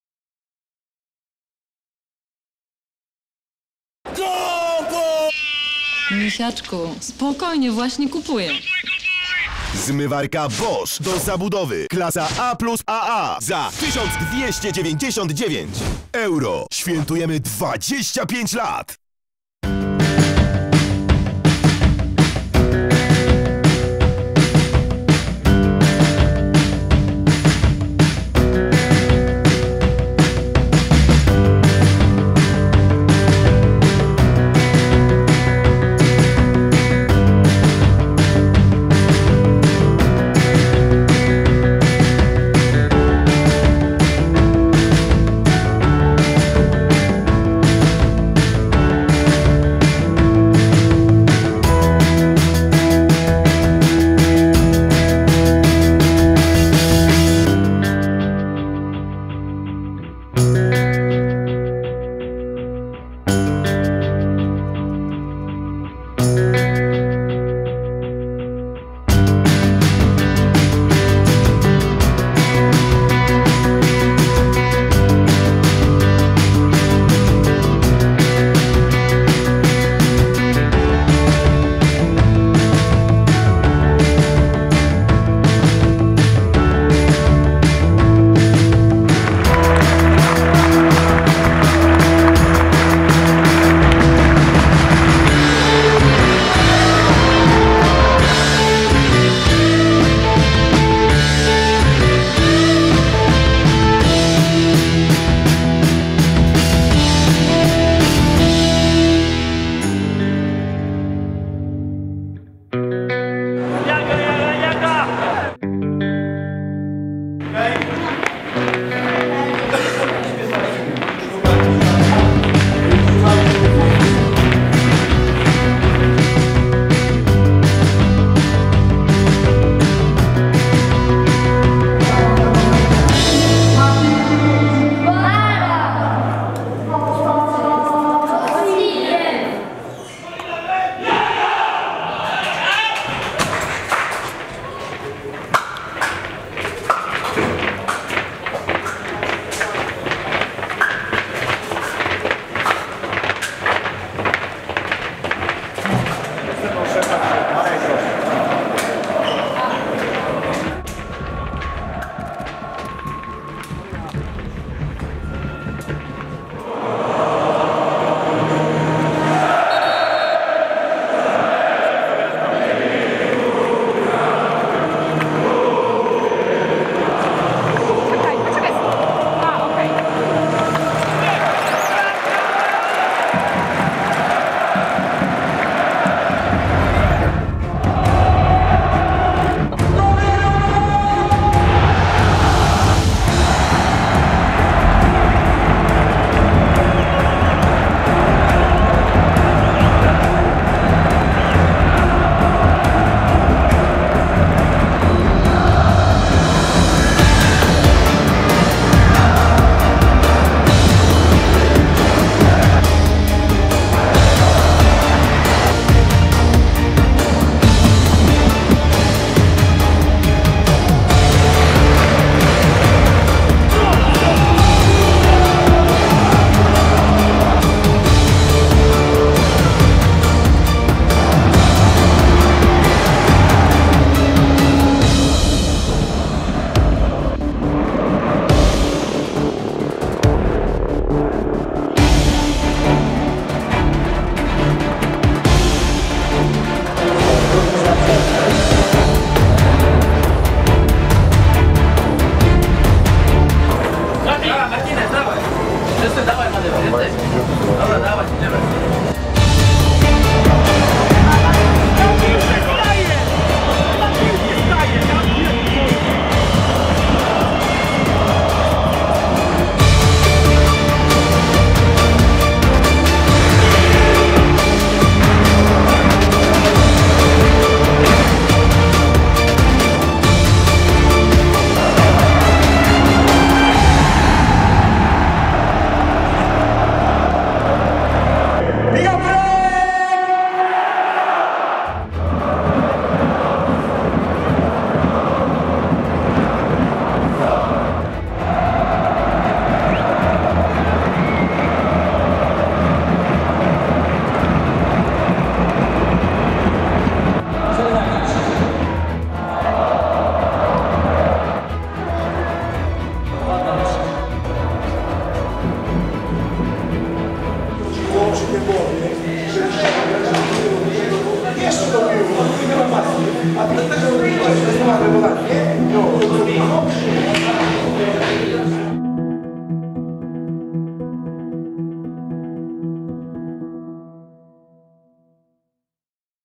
Wokół meczu Jaga vs Górnik
Po zwycięstwach nad Koroną i Zagłębiem, Żółto-Czerwoni zostali pokonani przez Górnika Zabrze. Przyjezdni uczcili swoje święto zwycięstwem, które ważyło się do ostatnich minut, kiedy rozstrzygającym trafieniem, piątym w tym tygodniu popisał się Roman Gergel.